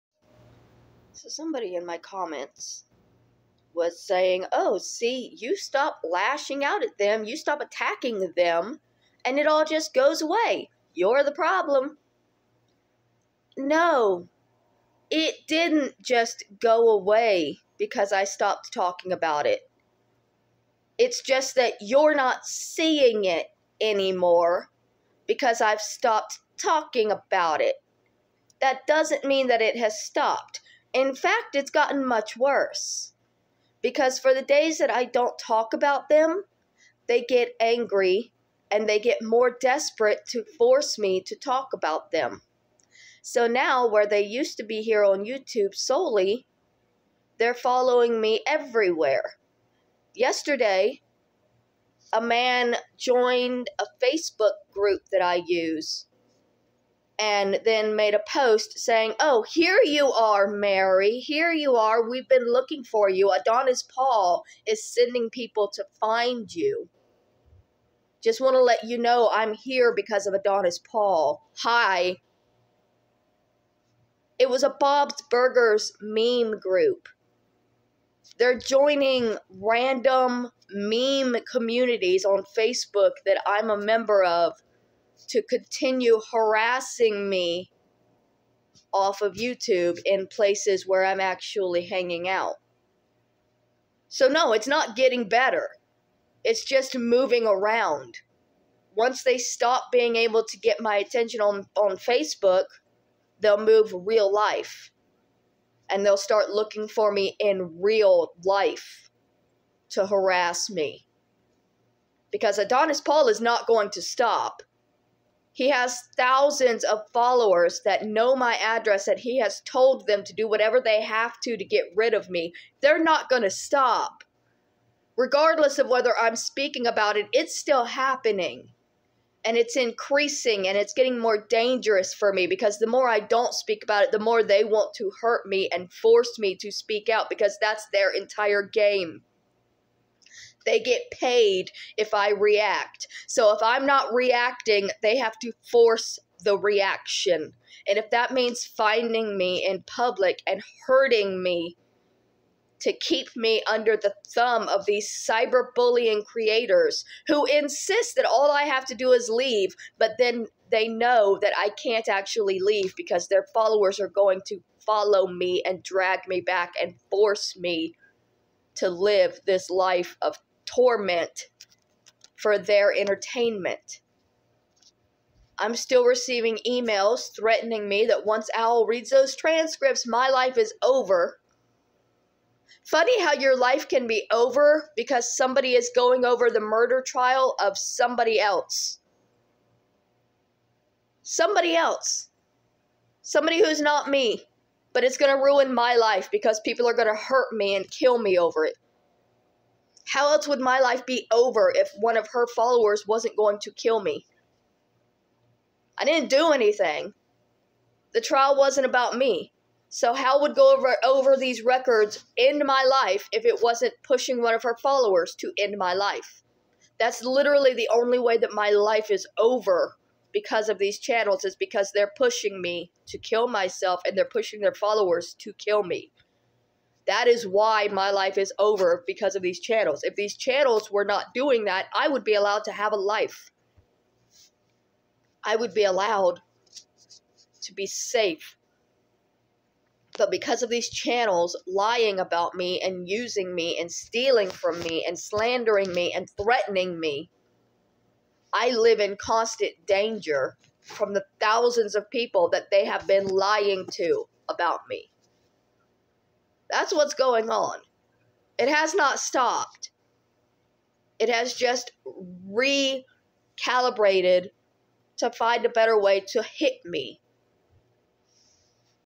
12/11/2023-1 - Leaving doesn't work they just find me and drag me back by lying that it worked
In this video Mary is very upset that nobody is showing her attention so she invents reasons to claim her imaginary "cyber bullying" has gotten worse.